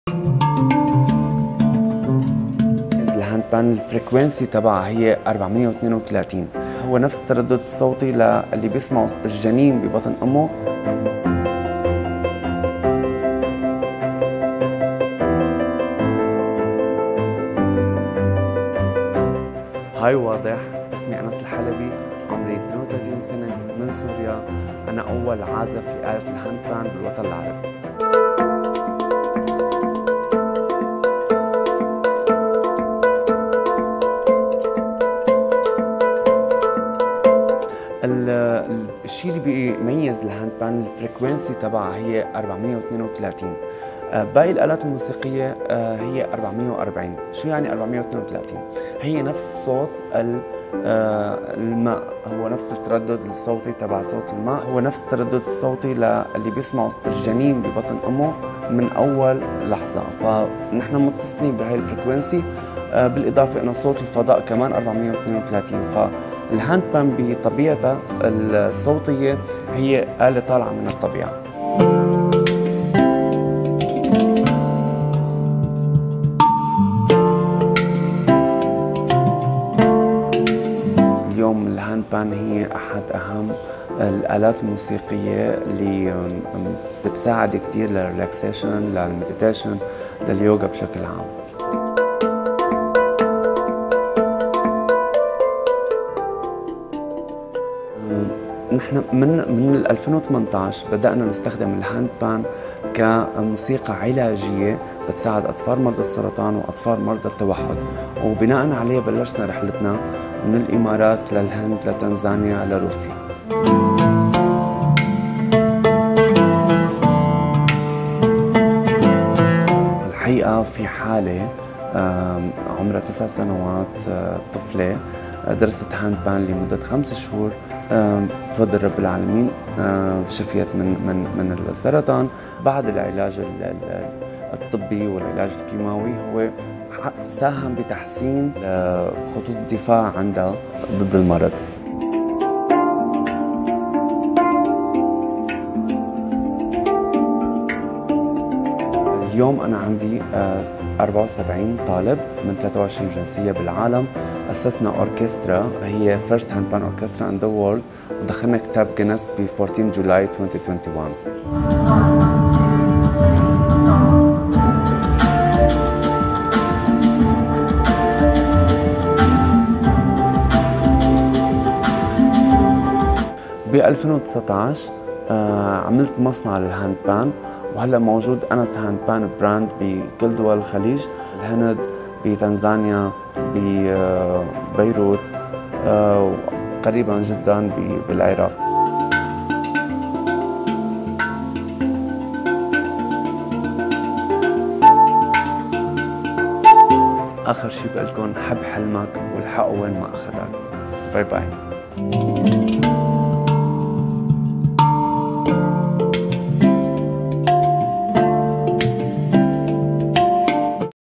Il est le 1er joueur arabe de handpan !
Ce Syrien est le premier joueur arabe de handpan, cet instrument de musique qui procure le bonheur !🎶

Wada7 Original